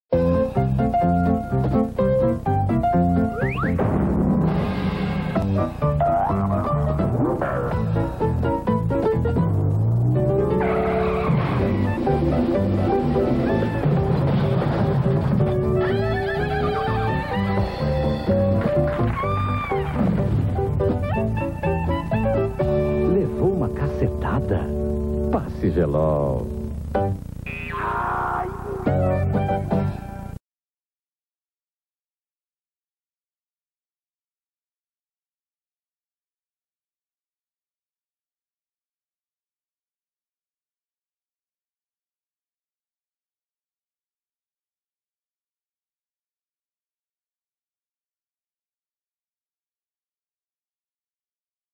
GELOL | Caiu, Bateu, Machucou? Passa Gelol que passa | 1992 | Comercial de TV
Comercial Histórico da TV Brasileira. Um dos mais lembrados. Com vídeo cassetadas que eram muito engraçadas.